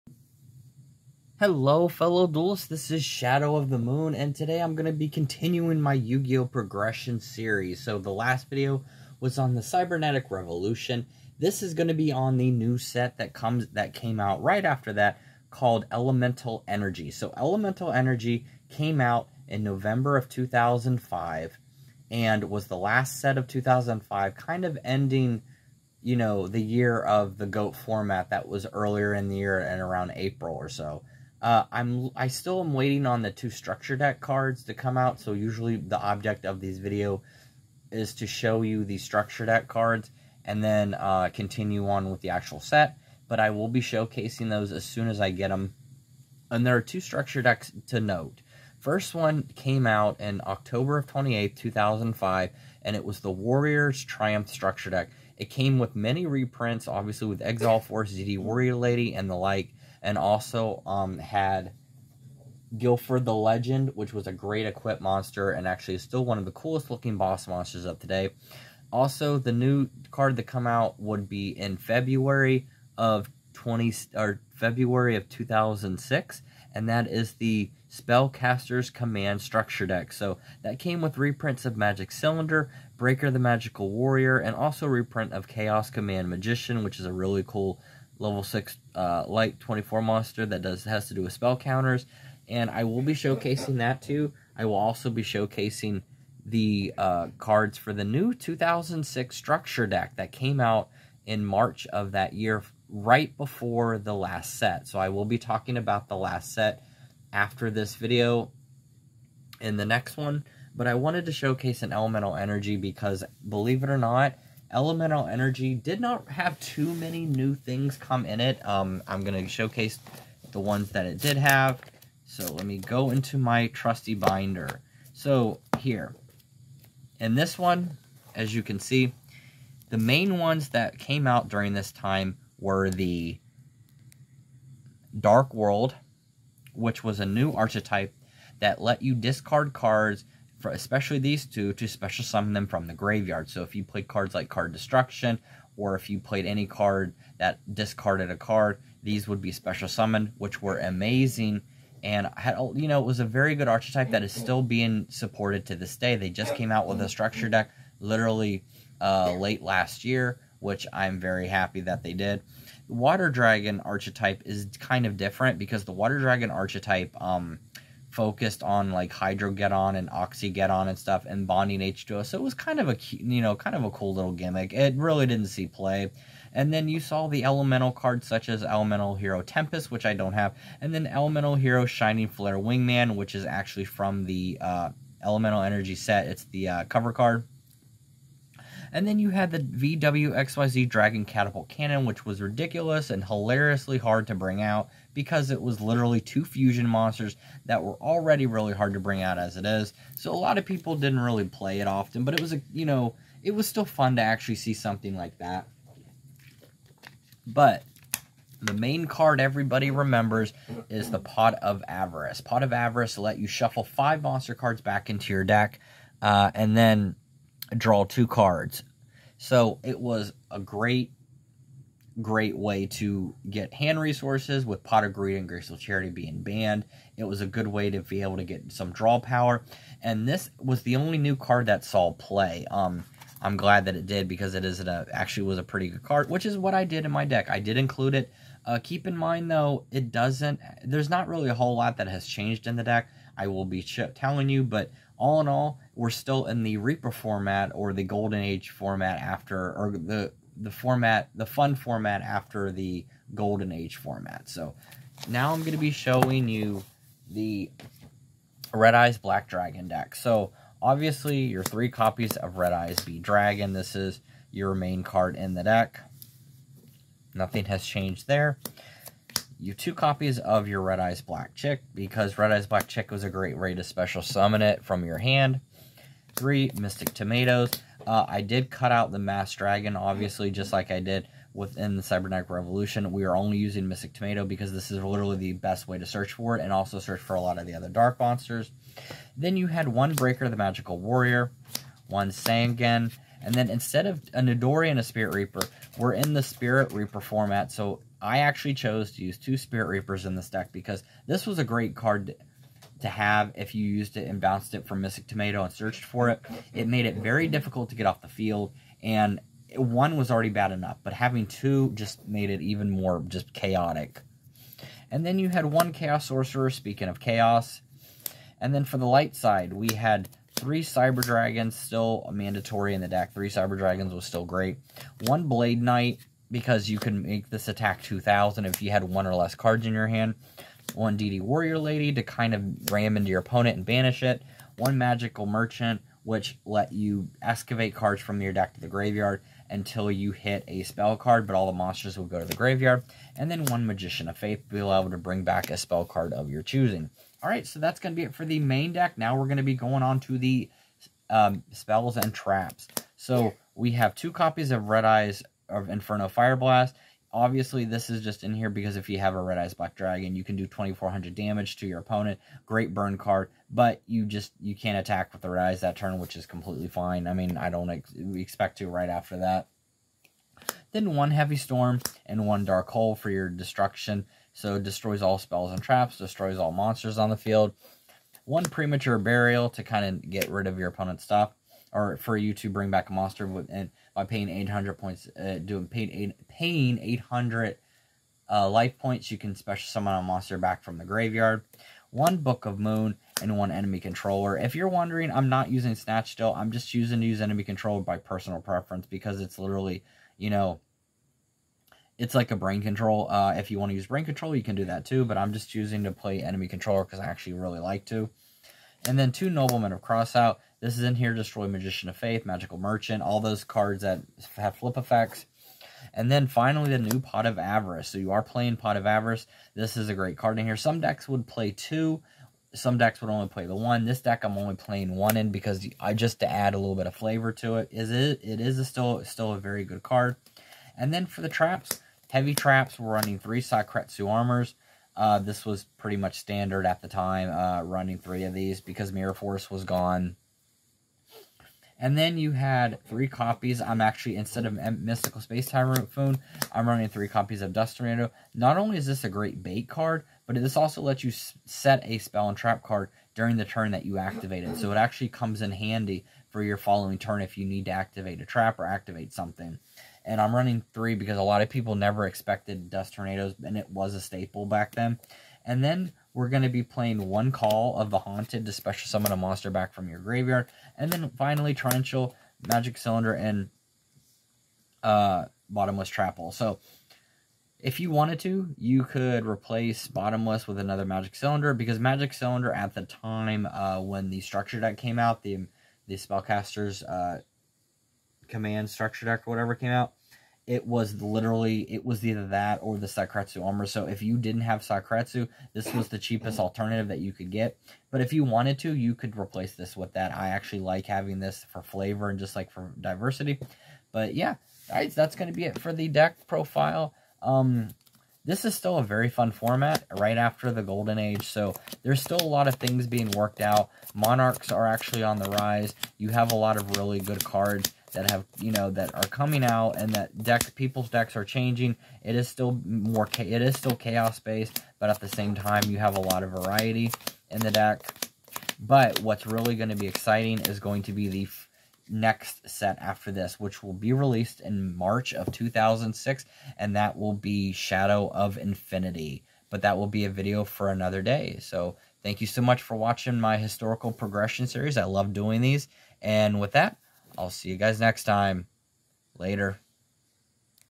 Yugioh Progression Series! Red-Eyes Black Dragon Deck Profile (Post-EEN)
My second episode in the progression series, spanning the entire gx era of Yugioh!  In this episode, we will be covering the Next set in the reaper format, Elemental Energy!